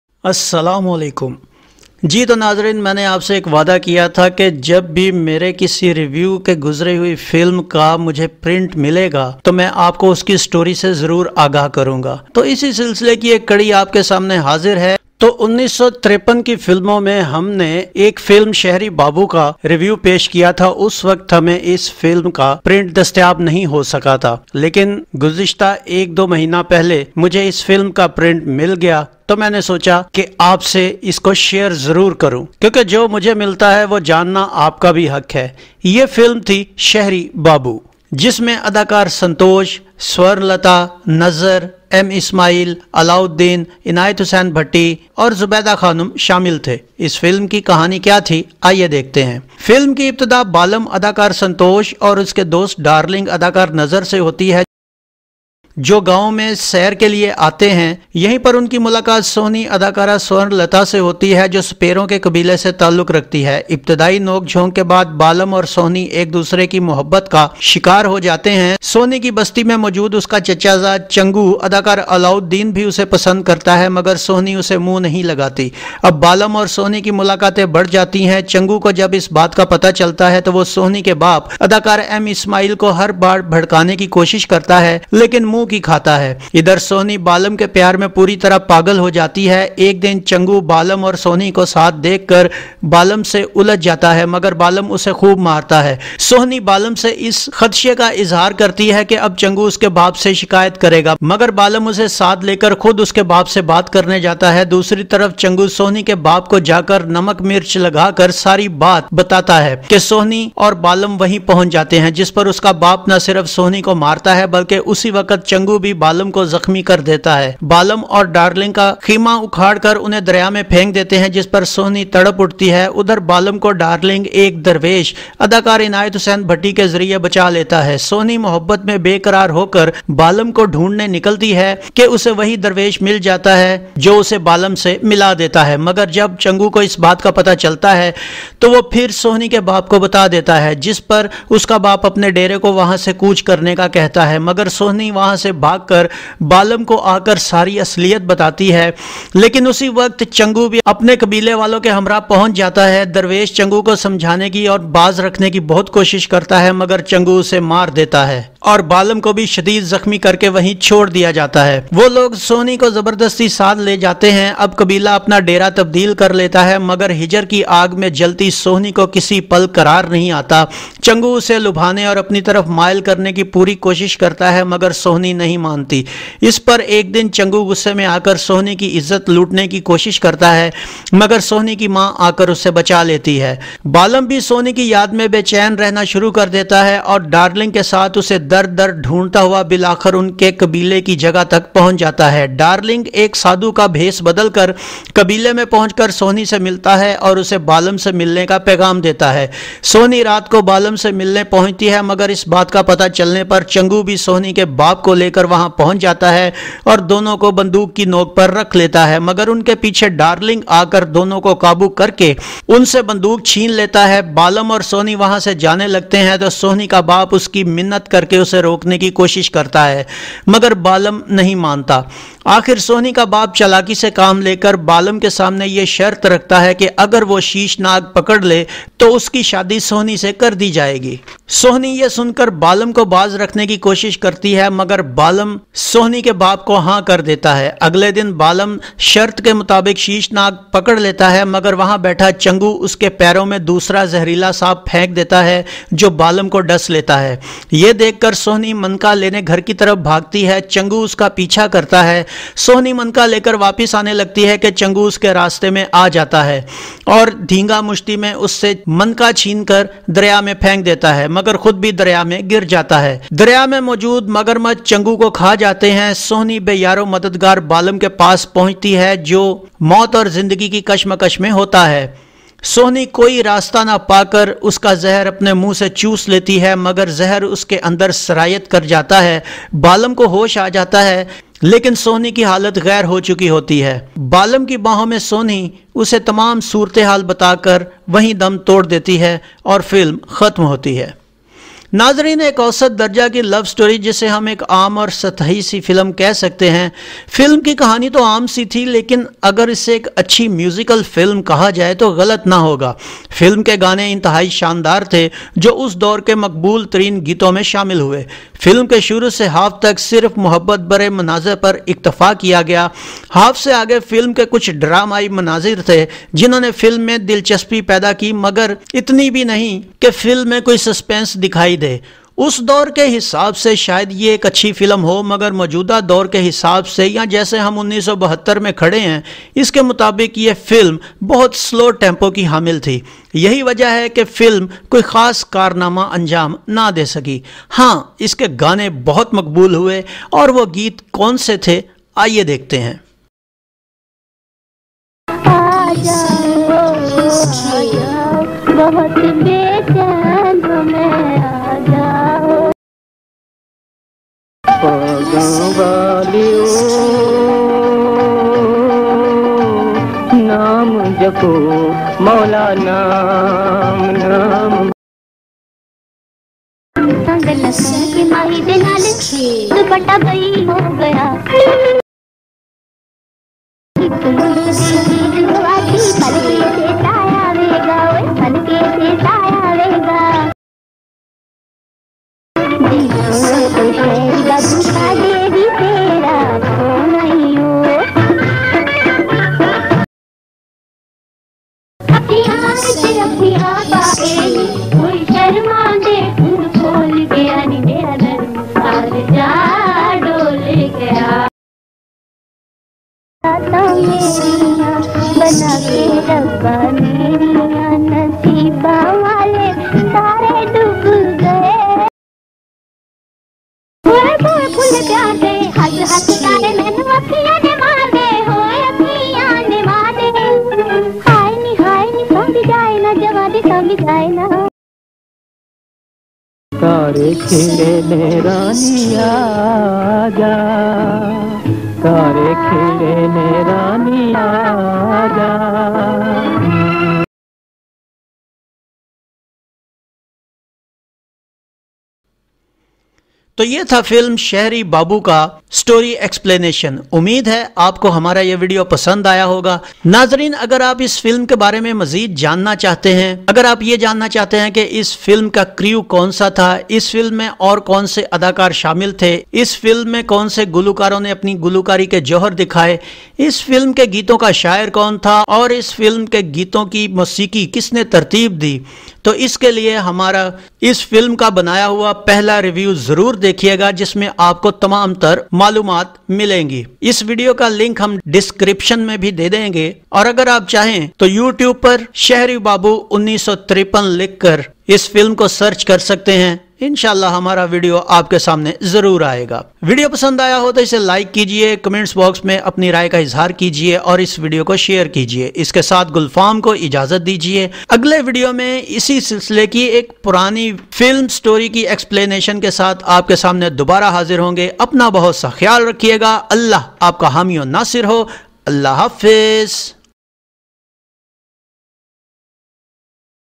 Shehri Babu 1953 | Story Explanation | Urdu/Hindi | Pakistani Classic Films | CRESCENT HISTORY
Today we Present a Brief story Explanation of 11nth Pakistani films in 1969 "Yaar Dost"
"Shehri Babu 1953" is the Social and Romantic film
its a Film of  "Santosh" and "Swarran Lata"
GULFAM   Pakistan GharnataShehri Babu
Santosh
Swarran Lata